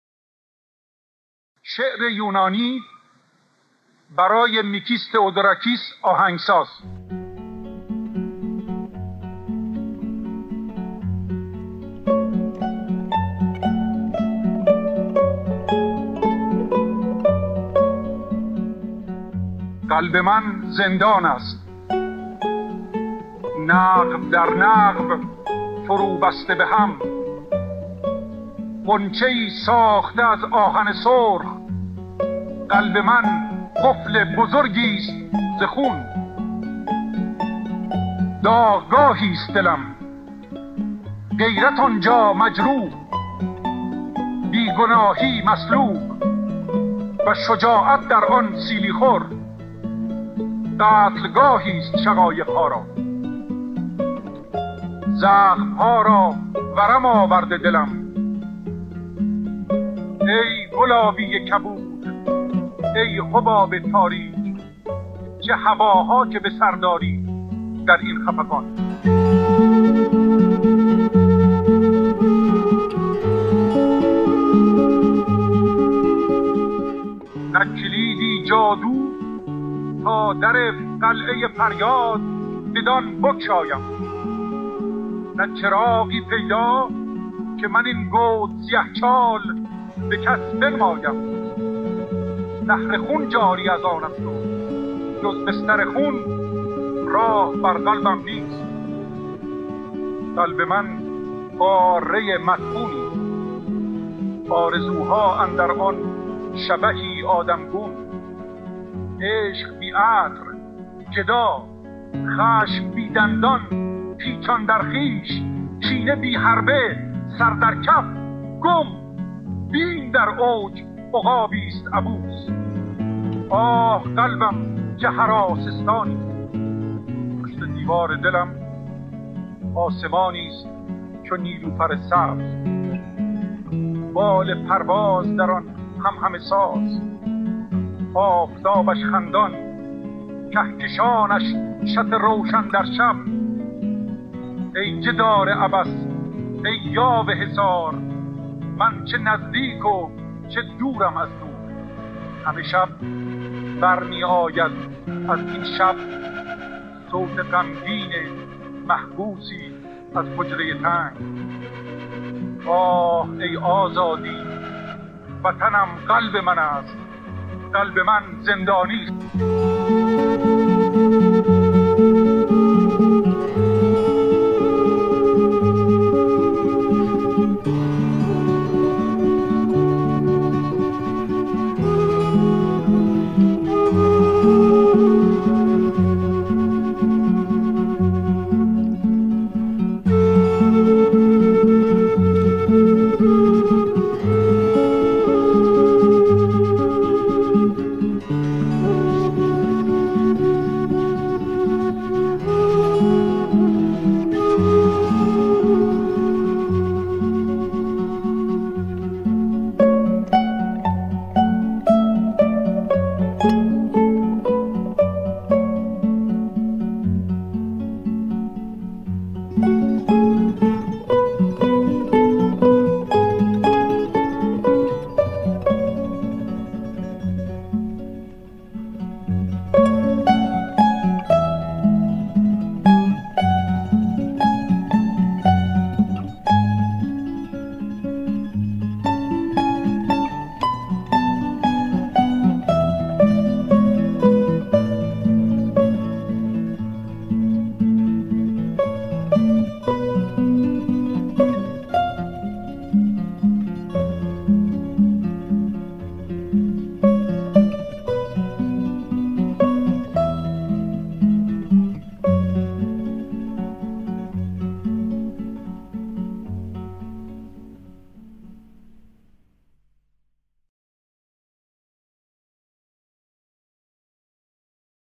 سیاوش کسرایی / شعر یونانی
شعر و صدا: سیاوش کسرایی
میکس: شاپور

شعر یونانی
برای:‌میکس تئودوراکیس آهنگ‌ساز
سیاوش کسرایی

قلب من زندان است
نقب در نقب، فرو بسته به‌هم
غنچه‌ای ساخته از آهن سُرخ
قلب من قفل بزرگی است ز خون

داغگاهی است دلم
غیرت، آنجا مجروح
بی‌گناهی مصلوب
و شجاعت در آن سیلی‌خور
قتلگاهی است شقایق‌ها را

زخم‌ها را ورم آورده دلم
پای تا سر همه قلبم، قلبم
می‌تپم، می‌تابم، می‌توفم
ای گلابی کبود!
ای حباب تاریک!
چه هواها که به سر داری در این خفقان!

نه کلیدی جادو
تا در قلعهٔ فریاد بدان بگشایم
نه چراغی پیدا
که من این گود سیه‌چال به کس بنمایم
نهر خون است و جز بستر خون
راه بر قلبم نیست

قلب من قارهٔ مدفونی‌ست
آرزوها اندر آن شبحی آدمگون
عشق، بی‌عطر، گدا
خشم بی‌دندان، پیچان در خویش
کینه، بی‌حربه، سر در کف، گم
بیم، در اوج، عقابی است عبوس
آه قلبم چه هراسستانی است.

پشت دیوار دلم
آسمانی‌ست چو نیلوفر سبز
بال پرواز در آن همهمه‌ساز
آفتابش خندان
کهکشانش شط روشن در شب
ای جدار عبث! ای یاوه حصار!
من چه نزدیک و چه دورم از نور.

همه شب بر می‌آید از این شب
صوت غمگین محبوسی از حجرهٔ تنگ:
آه ای آزادی
وطنم قلب منست
قلب من زندانی‌ست.

۱۳۴۷
از مجموعهٔ «چهل کلید»
پانویس:
ـ سرودهٔ «شعر یونانی»،‌ اولین بار با صدای خود شاعر در شامگاه شنبه ۲۳ مهر ماه ۱۳۵۶،‌ (شب ششم از  ده شب با شاعر ان و نویسندگان) در انجمن فرهنگی ایران ـ آلمان (گوته)، خوانده شد.